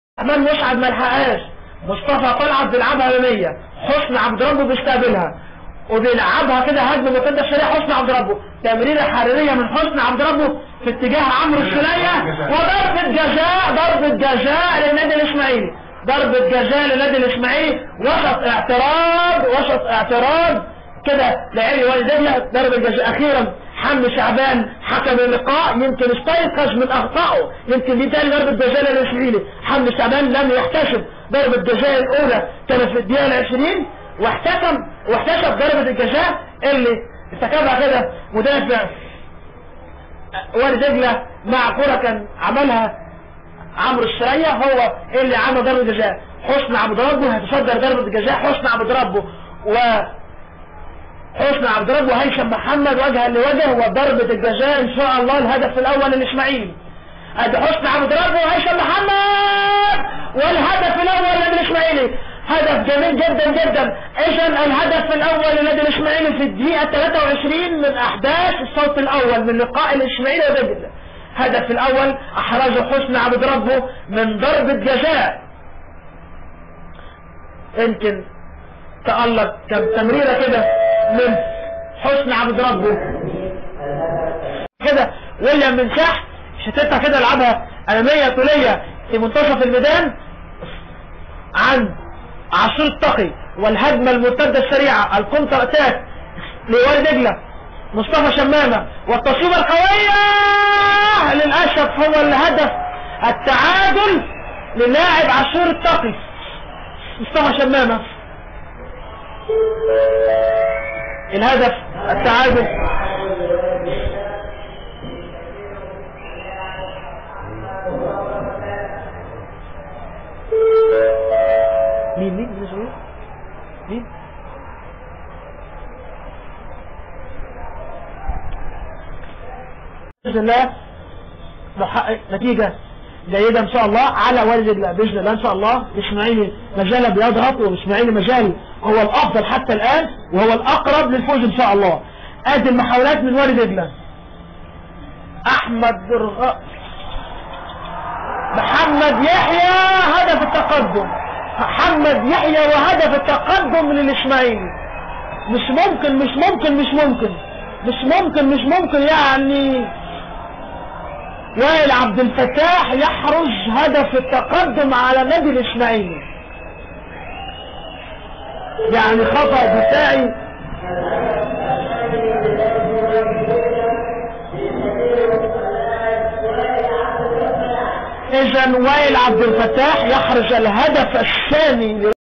اهداف وادي دجلة والاسماعيلي 3/7/2011
الاسبوع ال28
النتيجة 2 - 1 لصالح وادي دجلة
بكاميرا اسماعيلي 1924